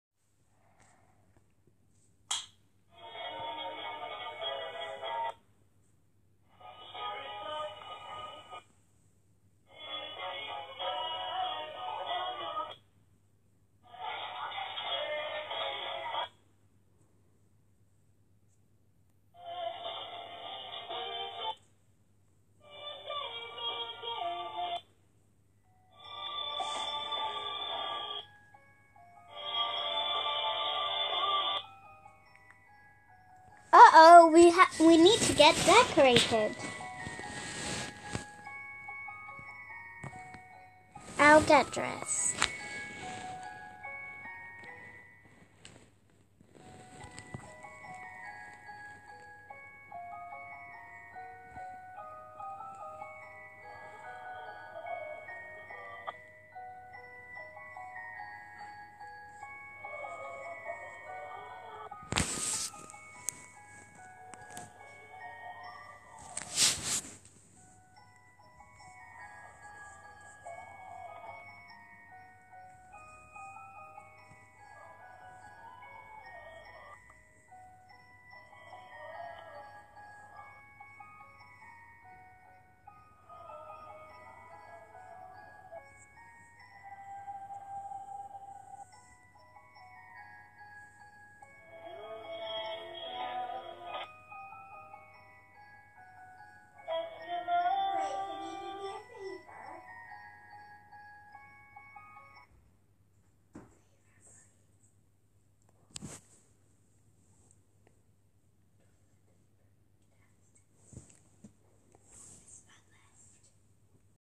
Anitales: "Tomorrow december!" - tntpuppy7
''Uh oh! We need to get decorated', tntpuppy7 said'. 'Tomorrow december!' created by tntpuppy7 using the Anitales app.